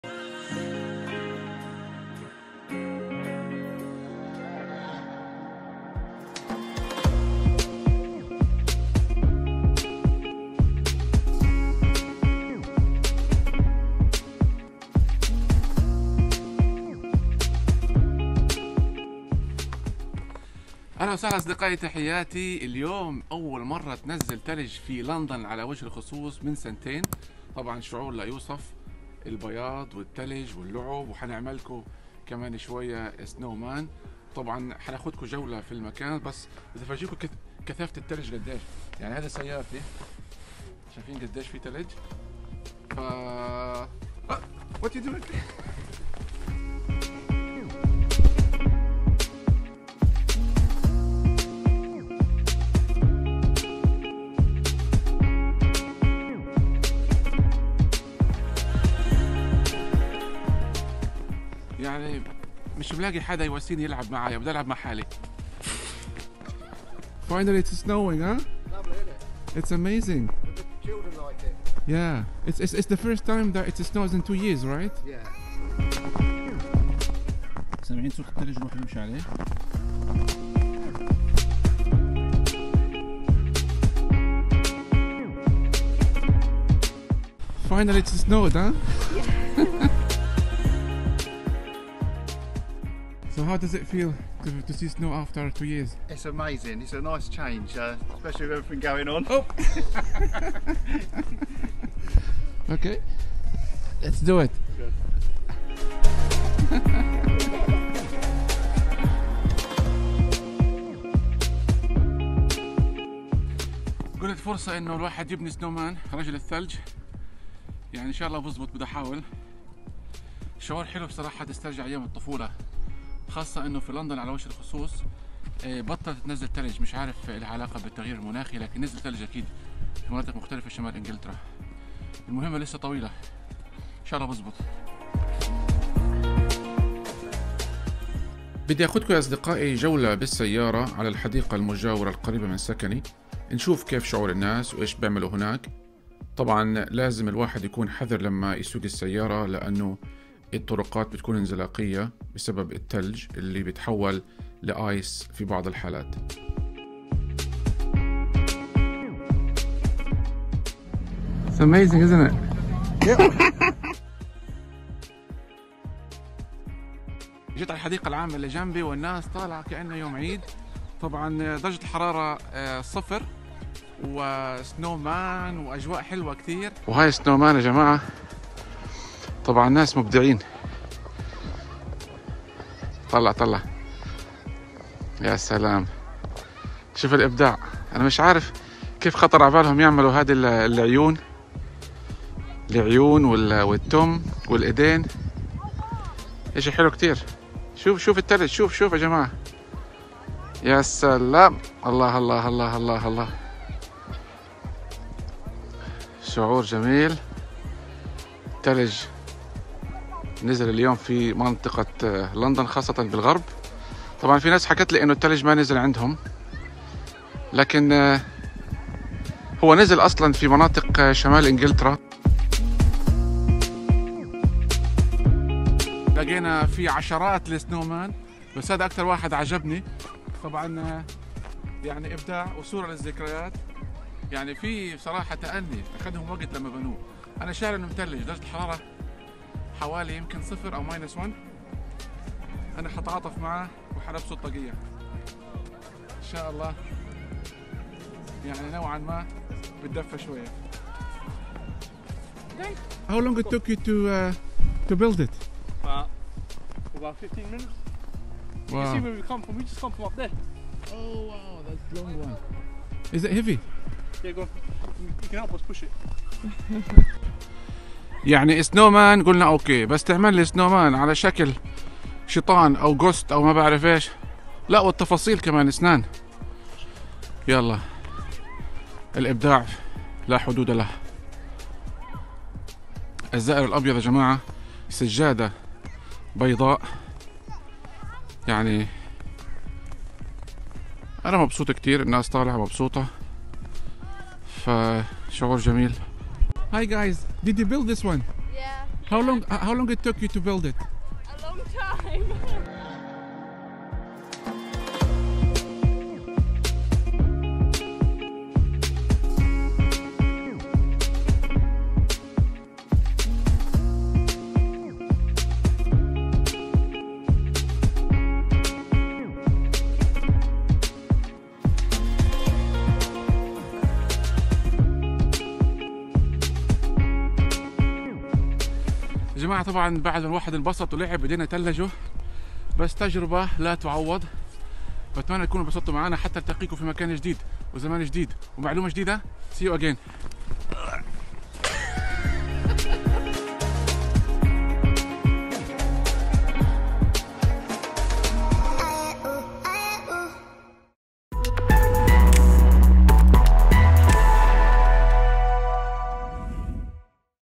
أخيرا نزل الثلج في لندن بعد انتظار دام سنتين ☃️❄️Snow in London 2021
Copyright to Mr Yousef Alhelou

Finally it snowed in London for the first time in 2 consecutive years! Pepole were really excited to see the white visitor amid the rules of lockdown in England.

أخيرا نزل الثلج في لندن بعد غياب دام سنتين.
بمجرد نزول الثلج خرج الناس الشوارع والحدائق العامة ولعبوا مع أطفالهم وبنوا رجل الثلج